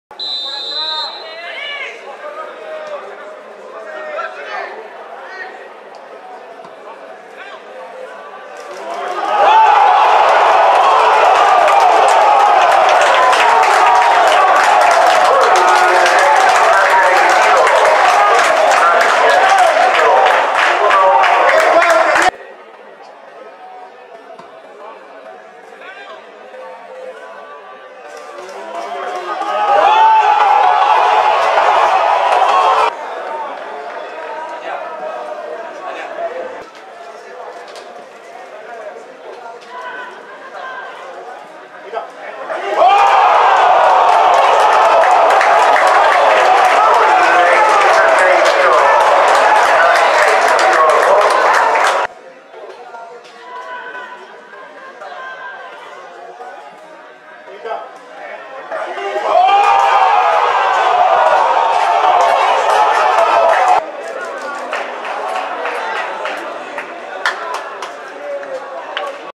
Gimnasia y Tiro (Salta) 2 vs. Gimnasia y Esgrima (Entre Ríos) 0
Torneo Federal A - Zona B
6ta. Fecha